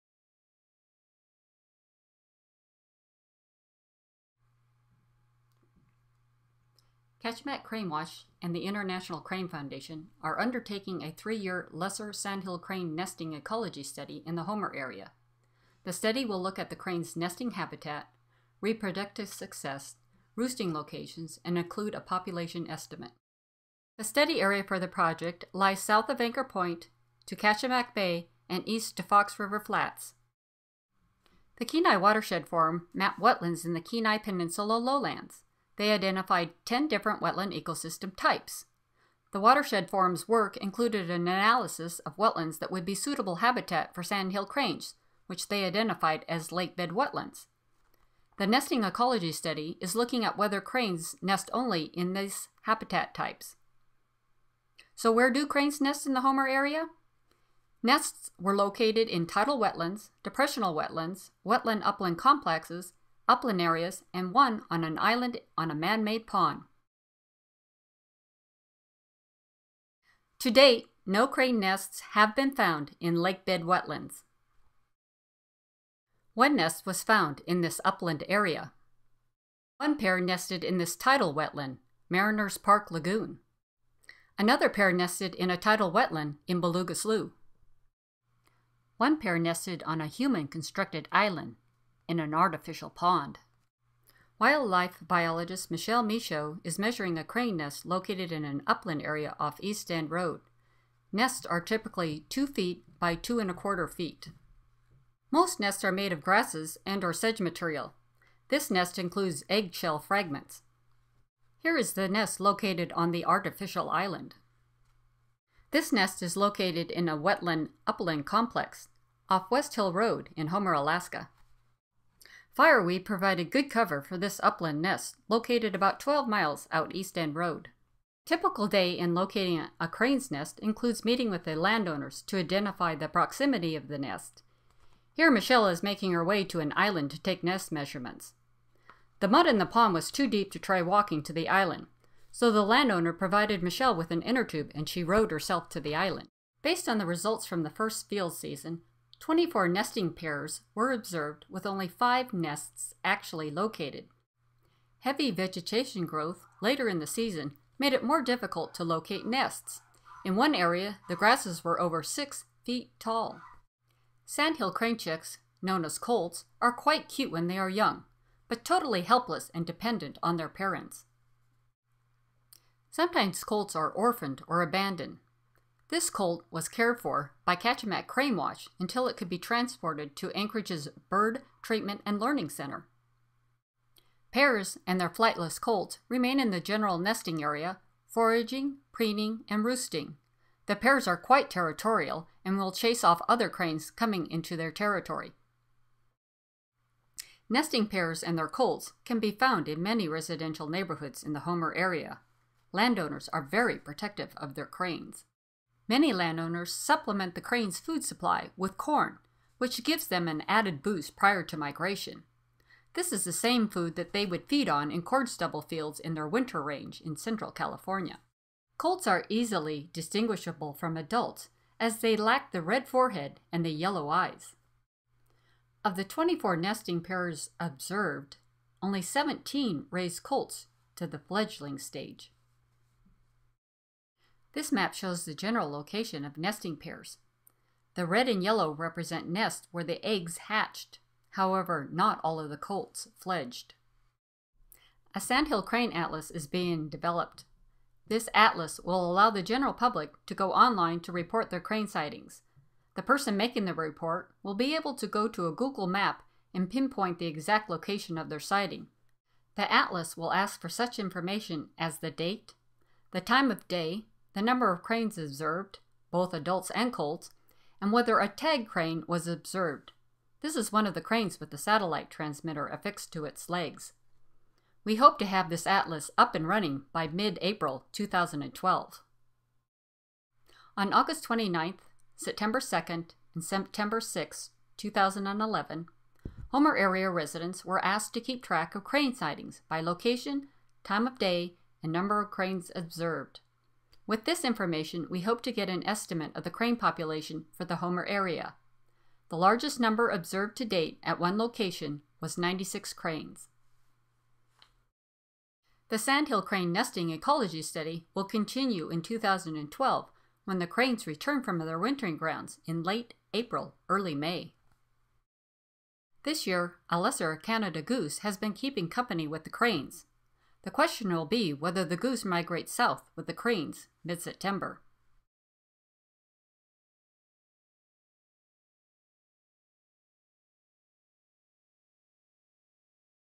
Sandhill Crane Nesting Ecology
Kachemak Crane Watch, in cooperation with the International Crane Foundation, is sponsoring a Sandhill Crane nesting ecology study in the Homer, Alaska area.  The end product of the three-year study will be a Sandhill Crane Nesting Atlas for the area.